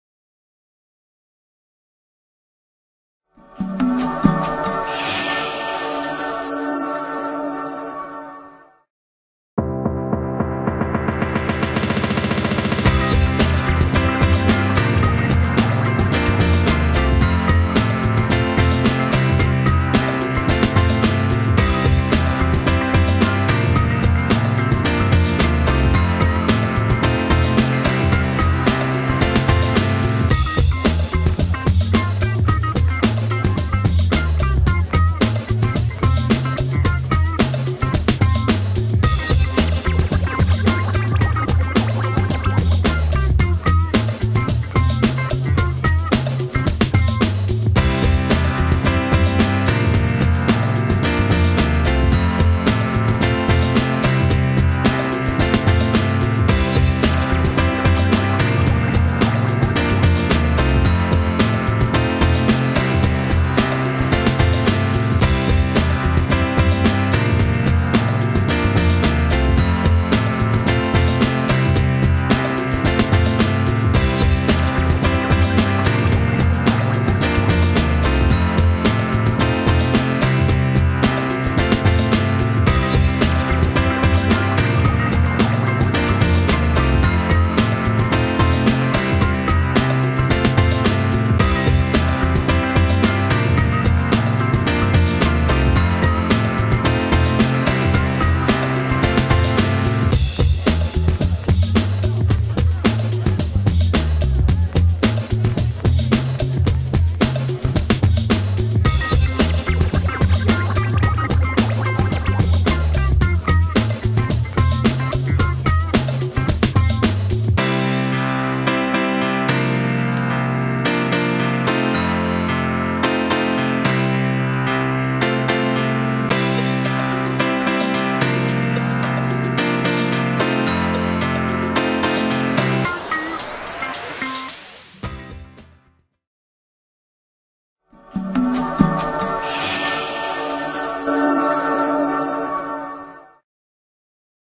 HOW TO SERIES Creation of Drainage Slope with Belzona 4154 and Belzona 5811
Belzona 4154 is a 2-part, multi-purpose epoxy based repair resin suitable for the rebuilding of concrete and stonework damaged by impact, vibration, chemicals, and environmental attack. Belzona 5811 is an epoxy coating designed for corrosion and chemical protection of equipment operating under immersion.